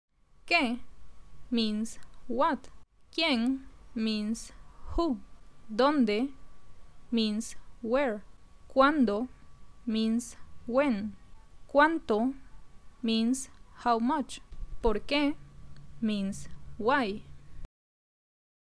How to say questions in spanish preguntas en español | Learning Spanish
Learning Spanish is so easy!
Today you will learn how to say questions in spanish

que means what in spanish
quien means who in spanish
donde means where in spanish
cuando means when in spanish
cuanto means how much in spanish